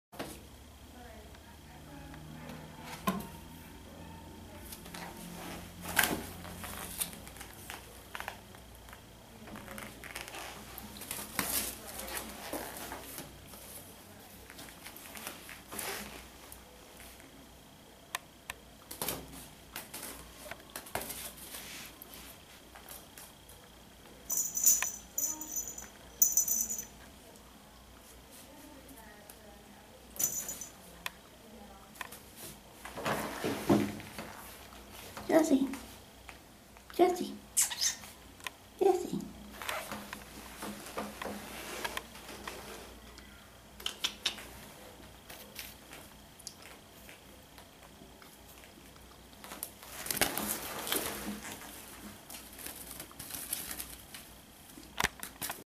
Meet Jessi a Domestic Shorthair currently available for adoption at Petango.com! 9/11/2015 9:42:45 A
Wow! I finally feel like my life is turning around for the better!  My name is Jessi and I am one of 5 kittens and one cat that were seized by the Humane Officer about 2 1/2 long months ago.  We were all abandoned in a house with no food or water.  Just last week we were at long last made officially the property of the Adams County SPCA and now we can be adopted!  I am only about 3 1/2 months old now and am still pretty shy.  When I came here I was just a tiny little kitten and they say it is lucky that I stayed healthy.  I was at first moved to one of the smaller condo rooms with two other kittens.  It was just too overwhelming for me and now they have me in the 'exotic room' in a big cage.  I feel safe in here and have been doing much better now. One of the volunteers had me out in the 'meet and greet' room yesterday and I got brave enough to play with toys and walk around the room!  Now I just want what everyone here wishes for every single day.  That is a home with a family that will love me forever!